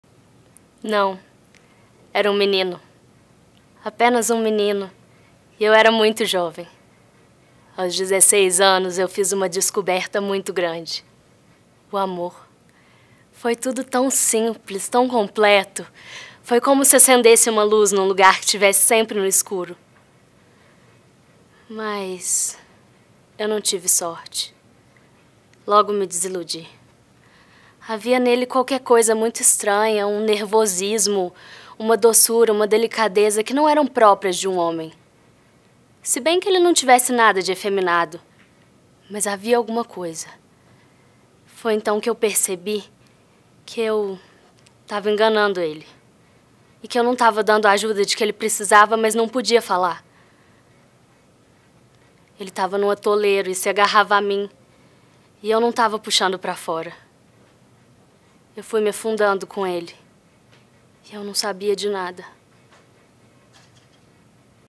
Gabriela Ananda - Monólogo
Atriz: Gabriela Ananda
DRT: 00405151/SP
Exercício de Monólogo - gravação com plateia (mínimo 30 pessoas).
Direção: Einat Falbel
Produção & Vídeo: Fco. Patrício/Casa de Artes
Local: Teatro Folha/Higienópolis/São Paulo/SP
Data: 28/Agosto/2017